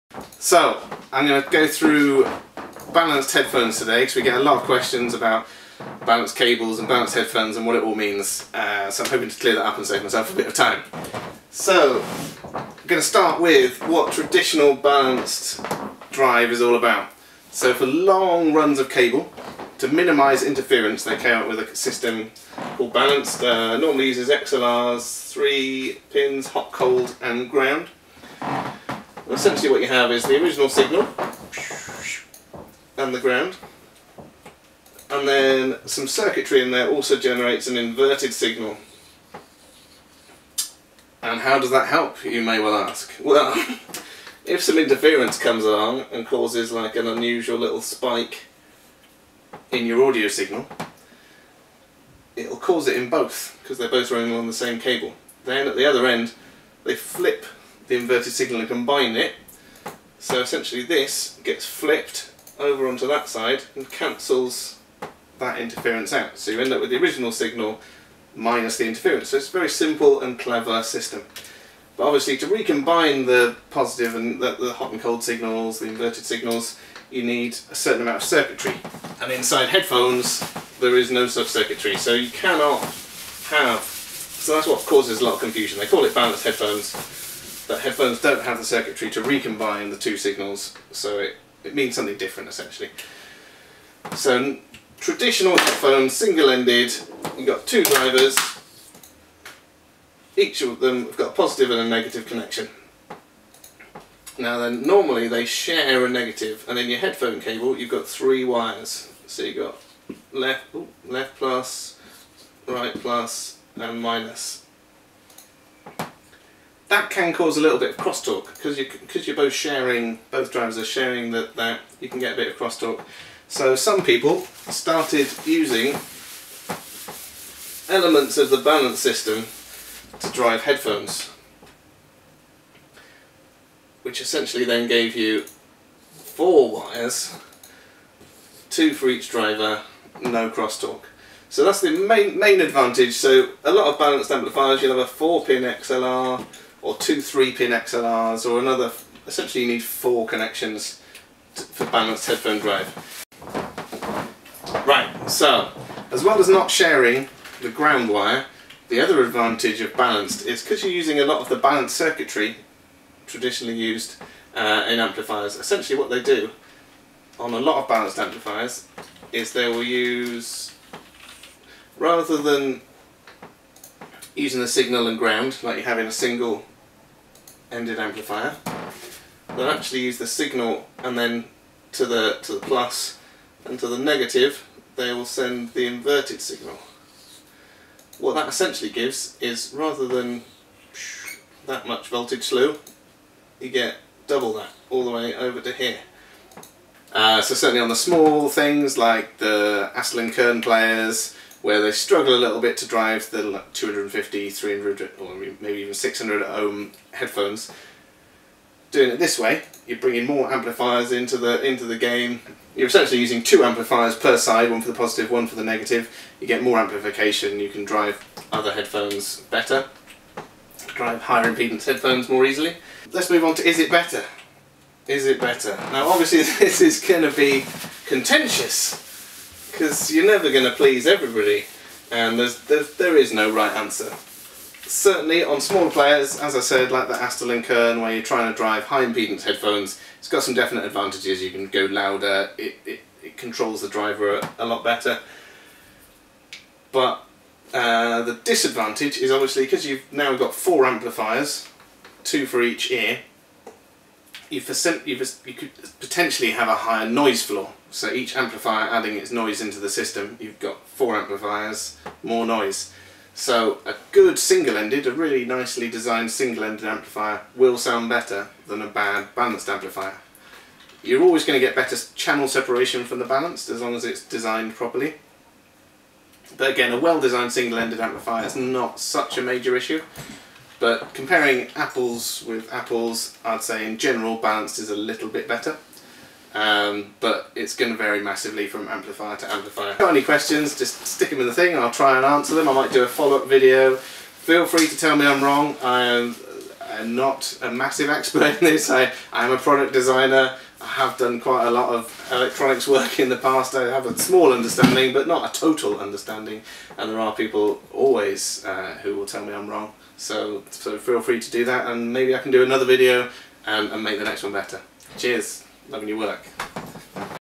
What is balanced headphone drive and how does it work
I get a lot of emails asking about balanced headphones, cables and amps and a lot of people are quite confused about what it means so to save me from responding with lots of lengthy emails I will be making a series of videos. Here is the first in the series explaining what balanced headphone drive means and a brief explanation of how it works